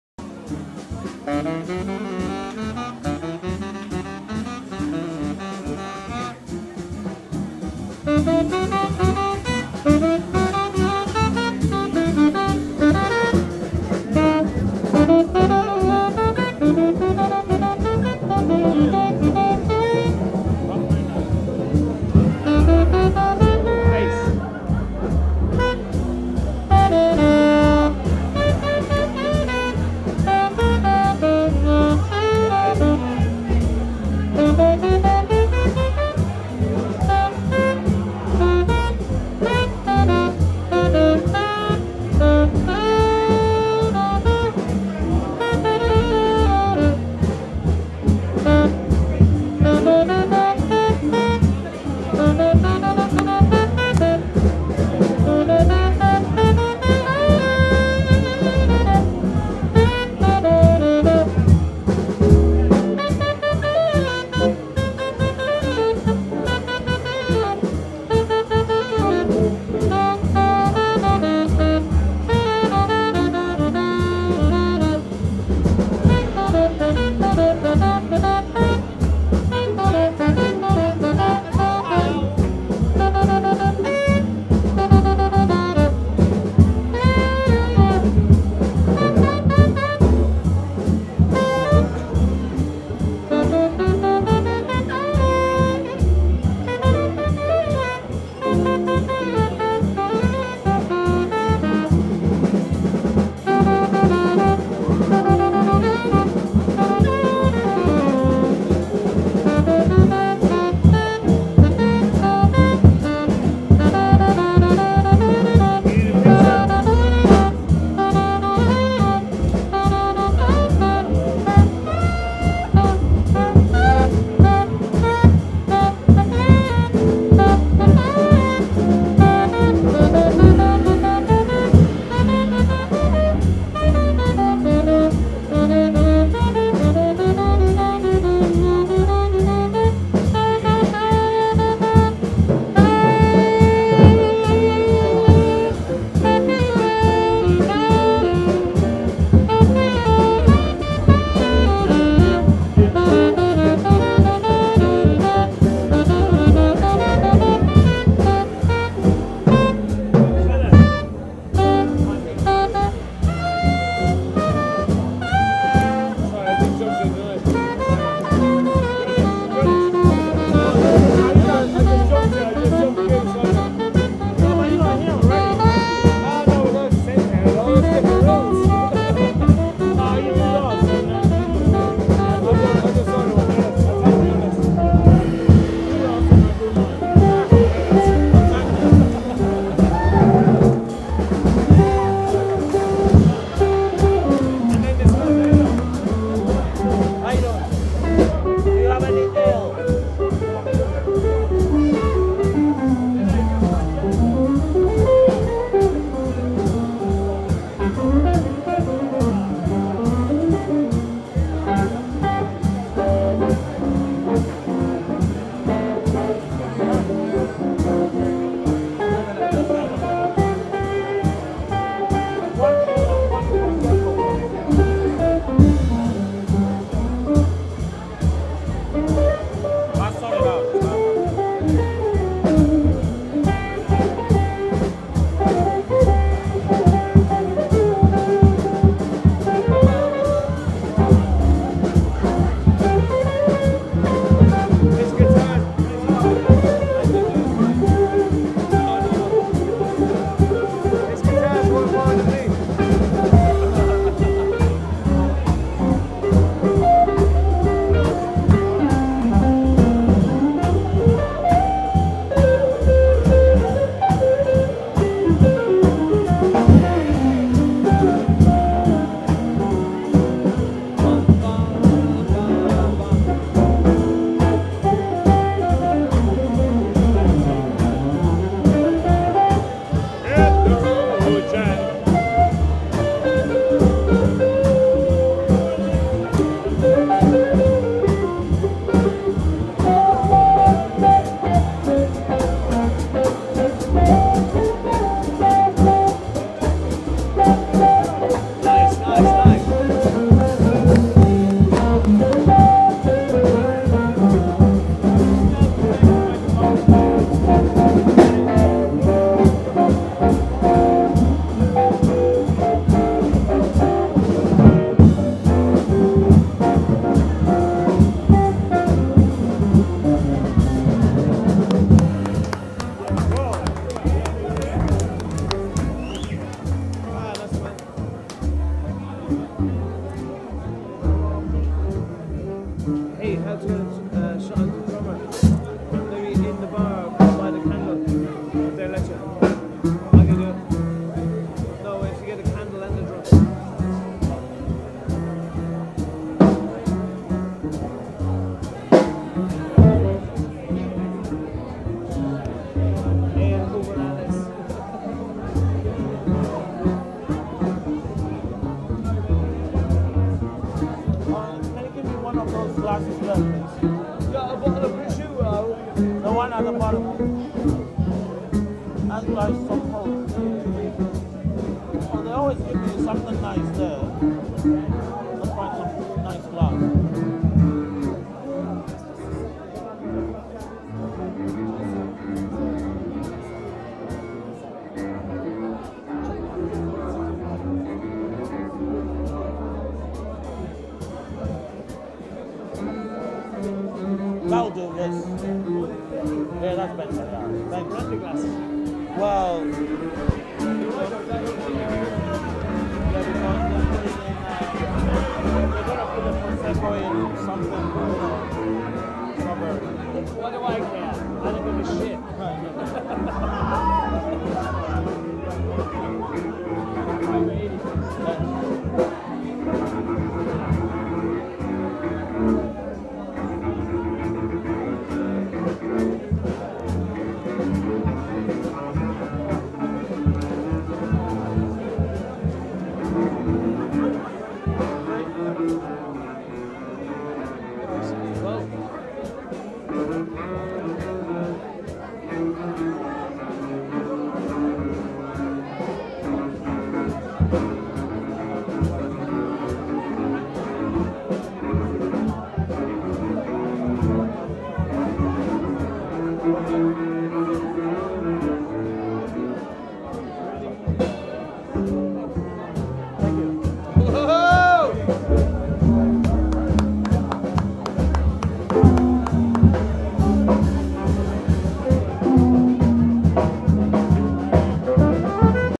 2014 03 02 Jazz at Haggerston Artie Zaitz, Musingi Brian Edwards, Ben Hazelton, Shane Forbes Hibla
Sunday at the Haggerston Pub, later in Dalston is so addictive. It is about 2:am. The Alan Weekes Quartet, Brian Edwards on Sax, Artie Zaitz Guitar, Shane Forbes Drums, Ben Hazelton Double Bass.
This is an image sound stream. The video is photographs and not in sync with the players. Enjoy, to the soon, Joseph